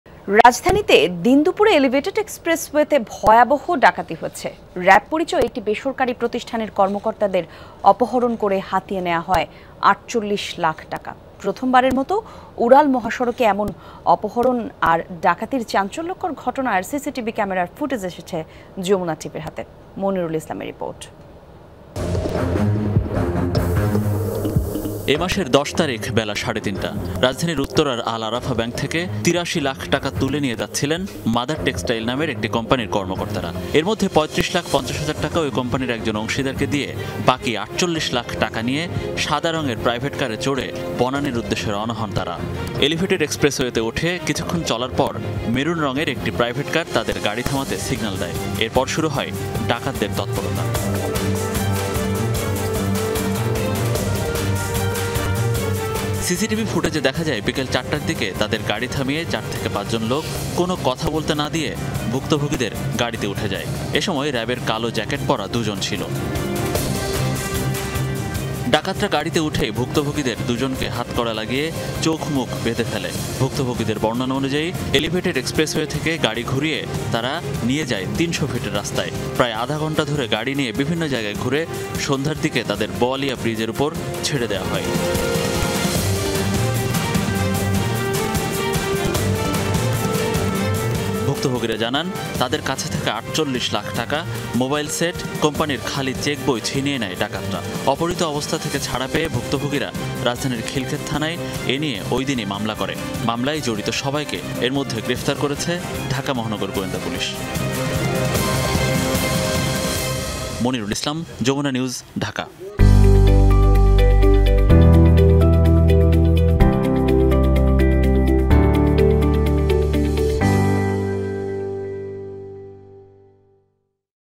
দিনে-দুপুরে এলিভেটেড এক্সপ্রেসওয়েতে দুর্ধর্ষ ডাকাতি; কী ঘটেছিল? | Expressway Robbery CCTV | JamunaTV
রাজধানীতে দিনদুপুরে এলিভেটেড এক্সপ্রেসওয়েতে ভয়াবহ ডাকাতি হয়েছে। র‍্যাব পরিচয়ে একটি বেসরকারি প্রতিষ্ঠানের কর্মকর্তাদের অপহরণ করে হাতিয়ে নেয়া হয় ৪৮ লাখ টাকা। প্রথমবারের মতো উড়াল মহাসড়কে এমন অপহরণ আর ডাকাতির চাঞ্চল্যকর ঘটনার সিসি ক্যামেরার ফুটেজ এসেছে যমুনা টিভির হাতে

Address:
Bangladesh.